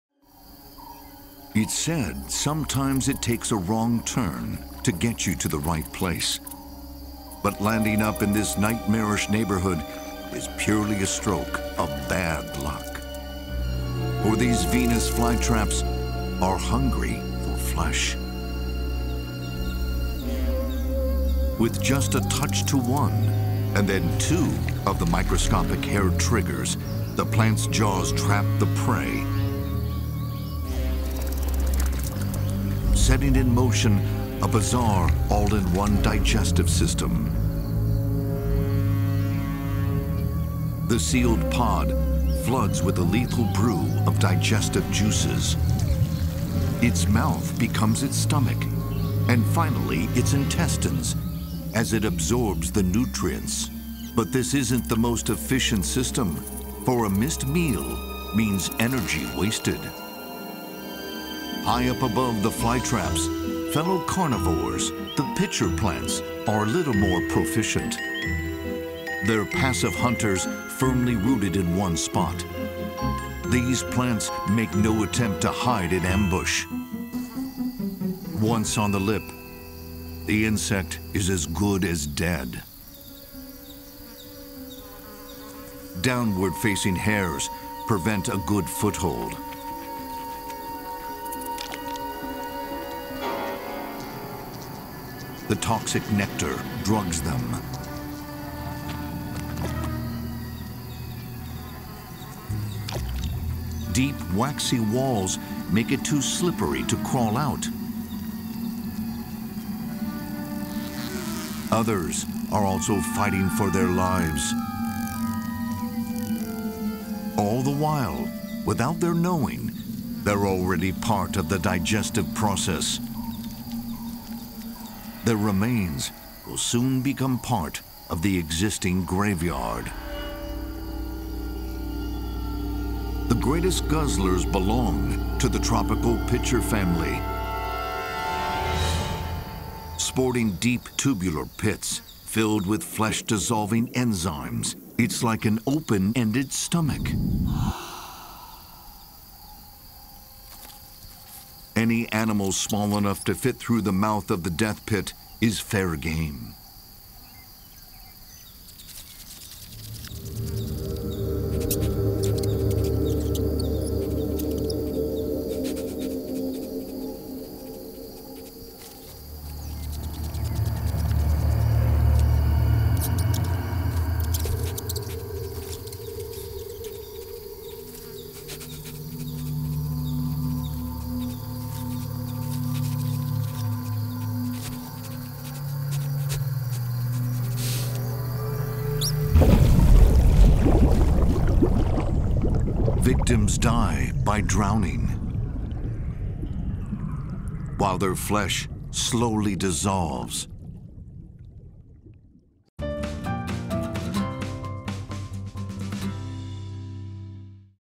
The Carnivorous Plant That Feasts on Mice
While the carnivorous cravings of most flesh-eating plants are limited to small insects, one exception is the pitcher plant. It can consume anything that fits in its mouth--including a mouse!